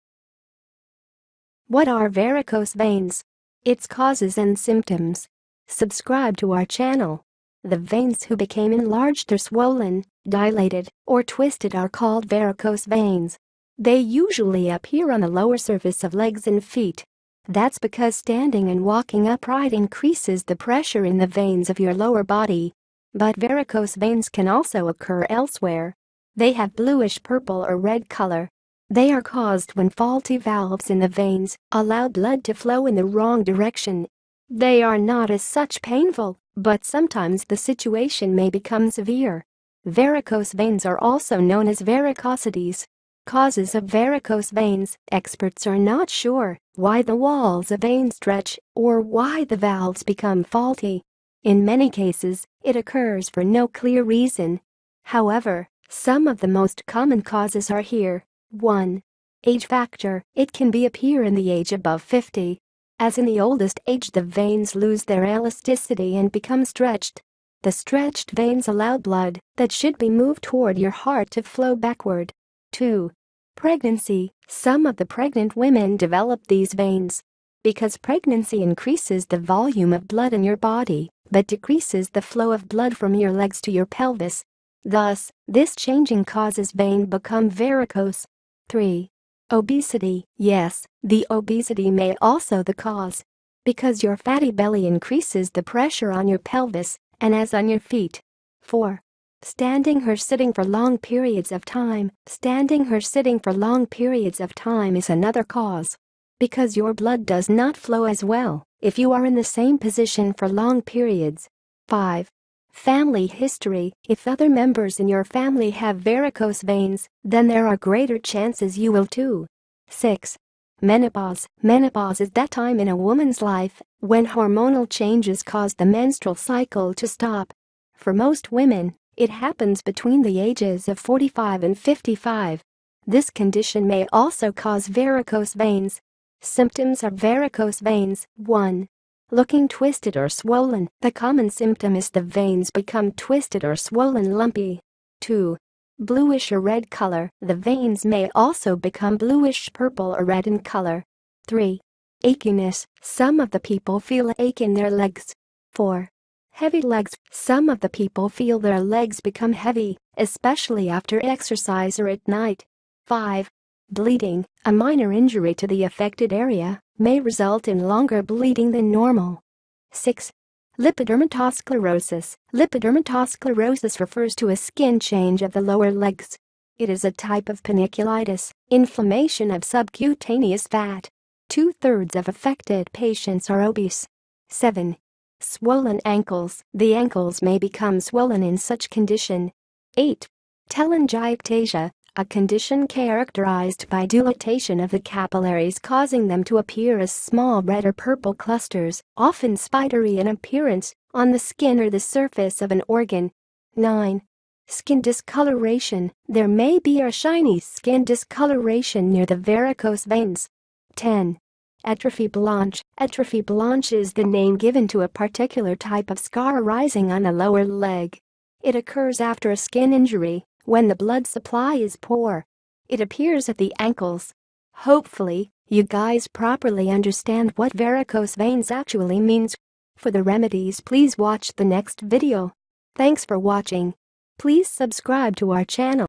What Are Varicose Veins? Its Causes And Symptoms
The veins that became enlarged or swollen, dilated or twisted are called varicose veins. They usually appear on the lower surface of legs and feet. That's because standing and walking upright increases the pressure in the veins of your lower body. But varicose veins can also occur elsewhere. They have bluish purple or red color. They are caused when faulty valves in the veins allow blood to flow in the wrong direction. They are not as such painful but sometimes the situation may become severe. Varicose veins are also known as Varicosities.
Causes OF Varicose Veins:
Experts are not sure why the walls of veins stretch or why the valves become faulty. In many cases, it occurs for no clear reason. However, some of the most common causes are here;
1. Age Factor:
It can be appear in the age above 50. As in the oldest age the veins lose their elasticity and become stretched. The stretched veins allow blood that should be move toward your heart to flow backward.
2. Pregnancy:
Some of the pregnant women develop these veins. Because pregnancy increases the volume of blood in your body, but decreases the flow of blood from your legs to your pelvis. Thus, this changing causes vein become varicose.
3. Obesity:
Yes, the obesity may also the cause. Because your fatty belly increases the pressure on your pelvis and as on your feet.
4. Standing or sitting for long periods of time:
Standing or sitting for long periods of time is another cause. Because your blood does not flow as well if you are in the same position for long periods.
5. Family History:
If other members in your family have varicose veins, then there are greater chances you will too.
6. Menopause:
Menopause is that time in a woman’s life when hormonal changes cause the menstrual cycle to stop. For most women, it happens between the ages of 45 and 55. This condition may also cause varicose veins.
Symptoms of Varicose Veins:
1. Looking Twisted or Swollen:
The common symptom is the veins become twisted or swollen lumpy.
2. Bluish or Red Color:
The veins may also become bluish purple or red in color.
3. Achiness:
Some of the people feel ache in their legs.
4. Heavy Legs:
Some of the people feel their legs become heavy, especially after exercise or at night.
5. Bleeding:
A minor injury to the affected area may result in longer bleeding than normal.
6. Lipodermatosclerosis:
Lipodermatosclerosis refers to a skin change of the lower legs. It is a type of panniculitis (inflammation of subcutaneous fat). Two-thirds of affected patients are obese.
7. Swollen ankles:
The ankles may become swollen in such condition.
8. Telangiectasia:
A condition characterized by dilatation of the capillaries causing them to appear as small red or purple clusters, often spidery in appearance, on the skin or the surface of an organ.
9. Skin Discoloration:
There may be a shiny skin discoloration near the varicose veins.
10. Atrophie Blanche:
Atrophie Blanche is the name given to a particular type of scar arising on the lower leg. It occurs after a skin injury when the blood supply is poor. It appears at the ankles.
Hopefully, you guys properly understand what Varicose Veins actually means?
For the remedies please watch the next video.